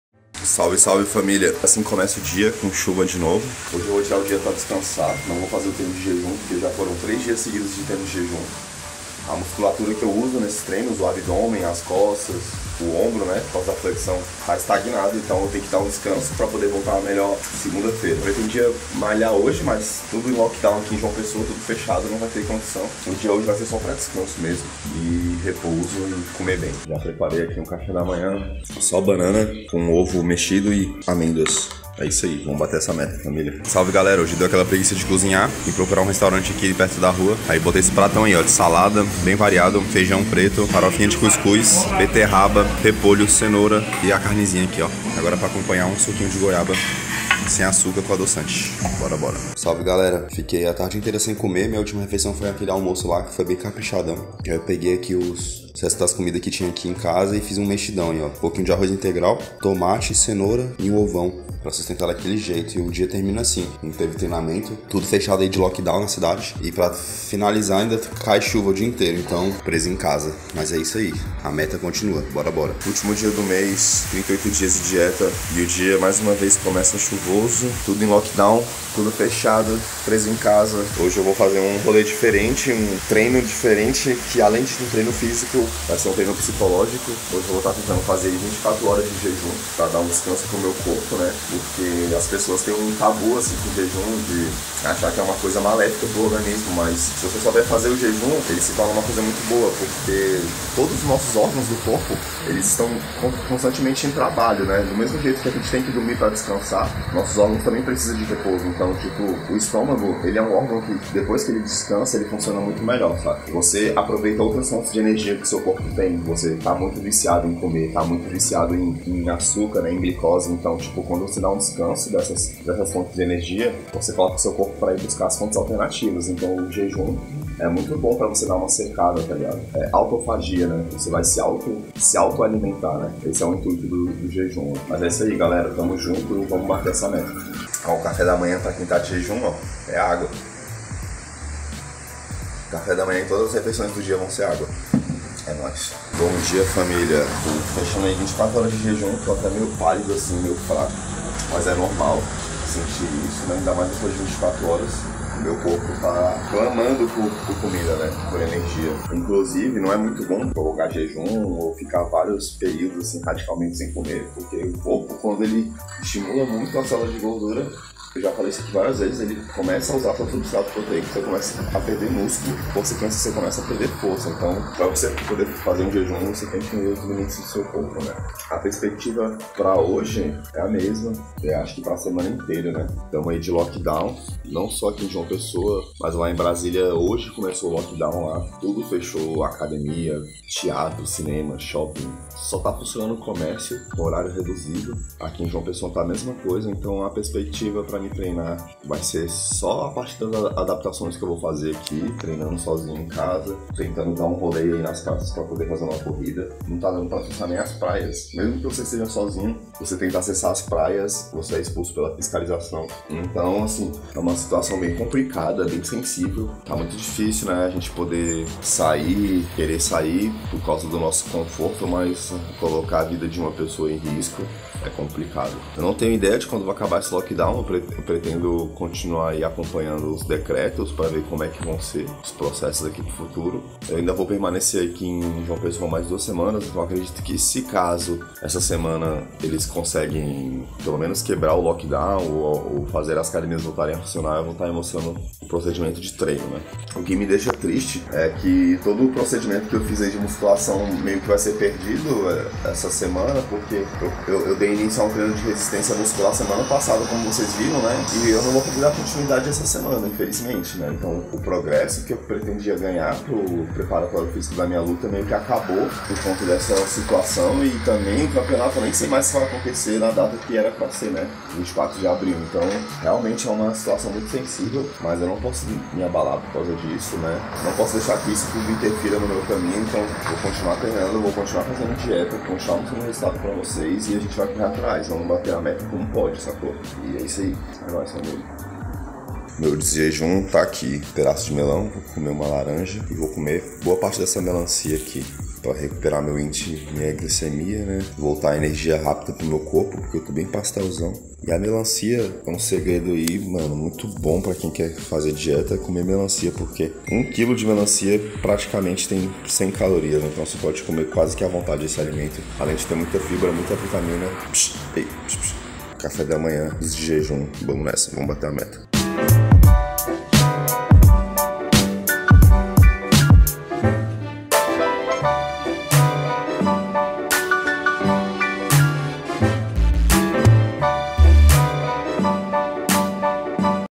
3 DIAS TRANCADO EM CASA SEM PODER TREINAR (DIA 37, 38 e 39)
Por conta do lockdown e intensos dias chuvosos eu tive que ficar em casa sem treinar e para compensar fiz 24h de jejum. Acompanhe aí como foram esses dias.